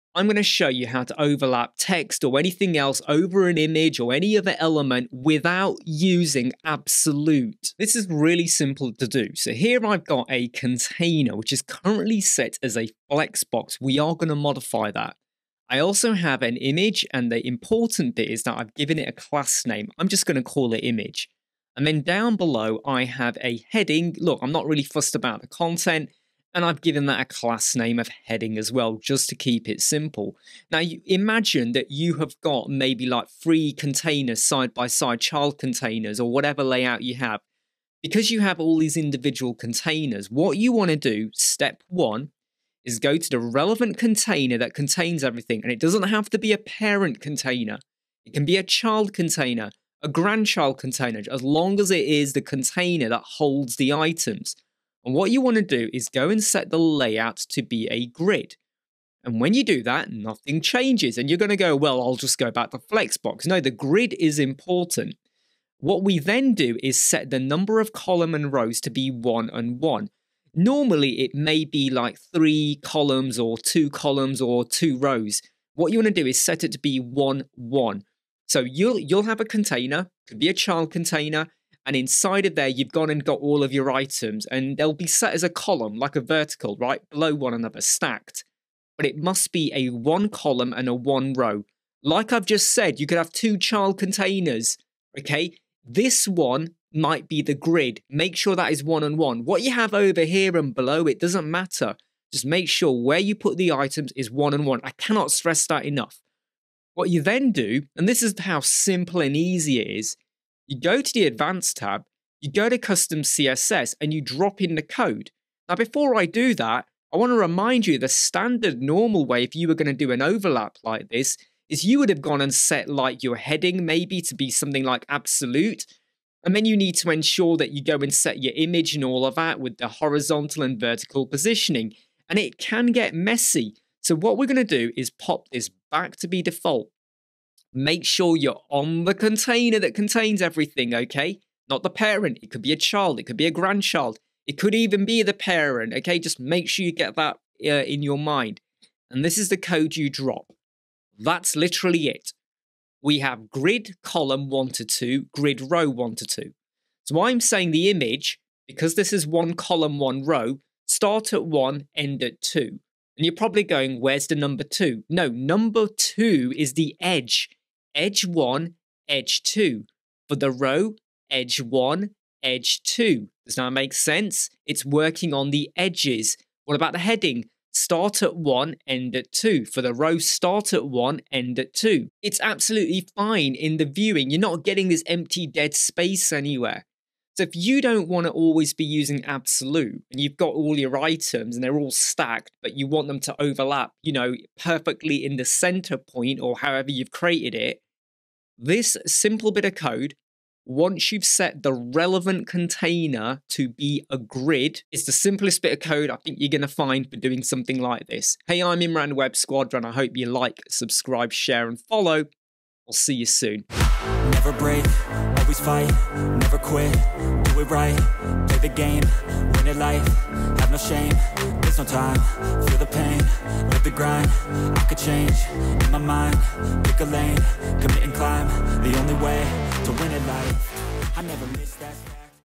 Overlap Elements without Absolute - Use CSS and Grid - Elementor Wordpress Tutorial
CSS:
.image {
    grid-column: 1 / 2;
    grid-row: 1 / 2;

.heading {
    grid-column: 1 / 2;
    grid-row: 1 / 2;